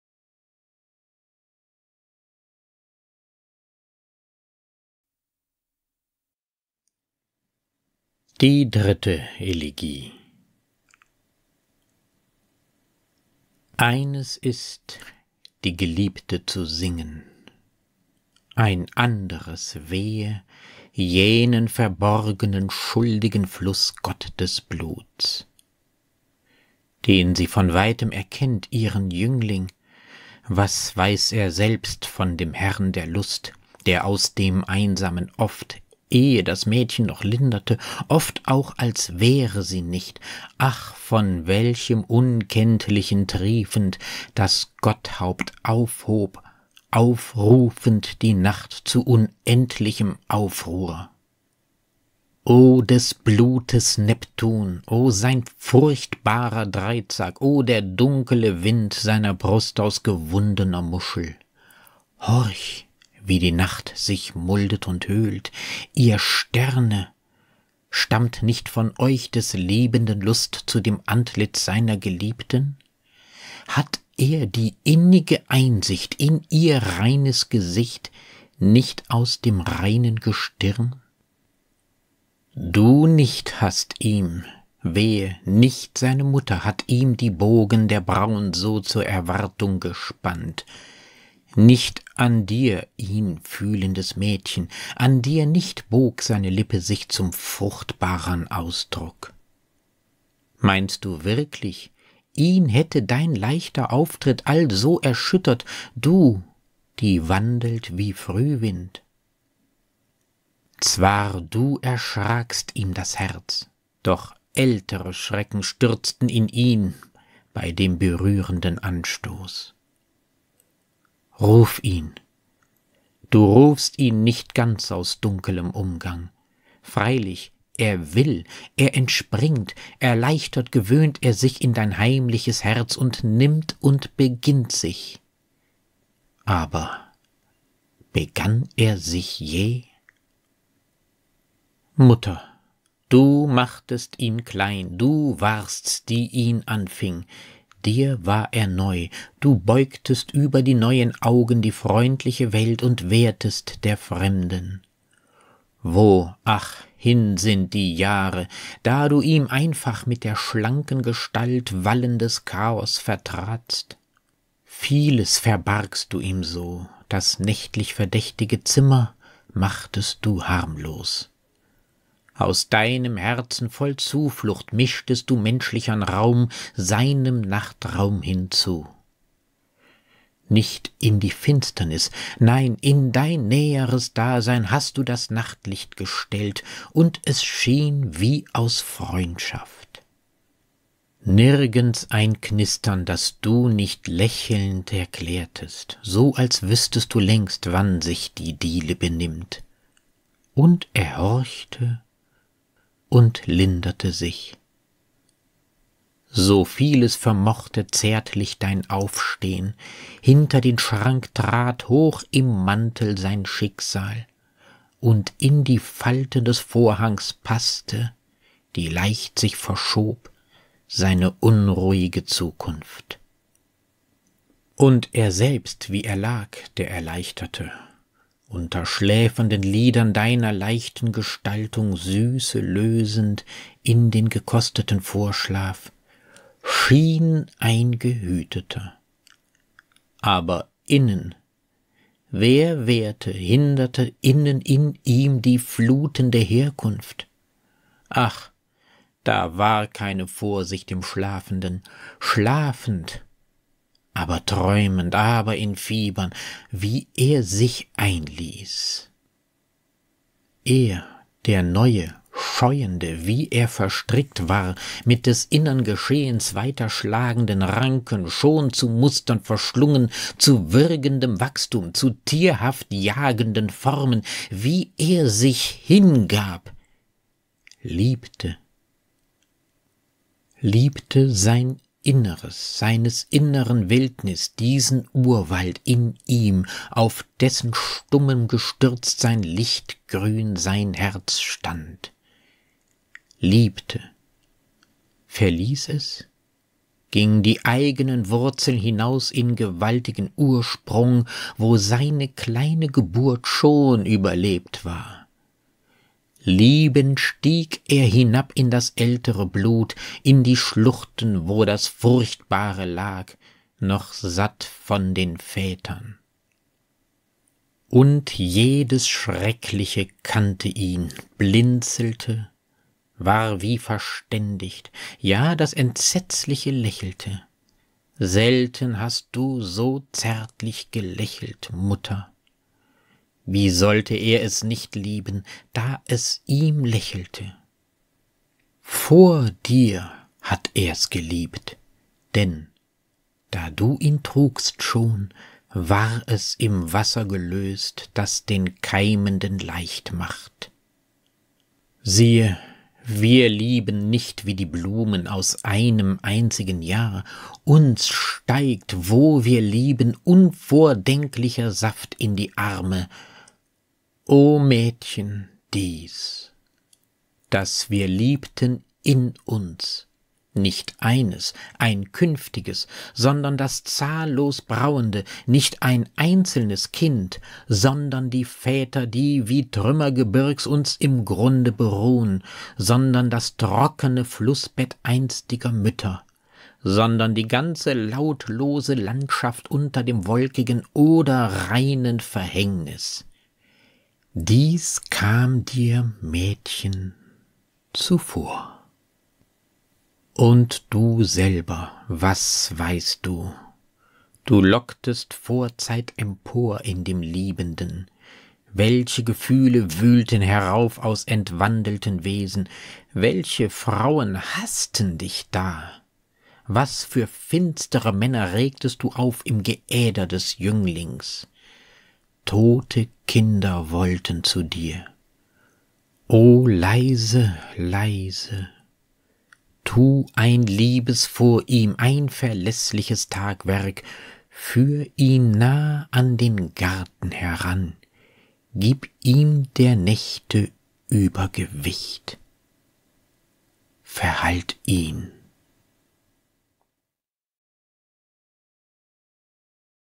Rainer Maria Rilke: Die dritte Elegie (Duineser Elegien)
Liebe, Triebe und Psychoanalyse poetisch reflektiert.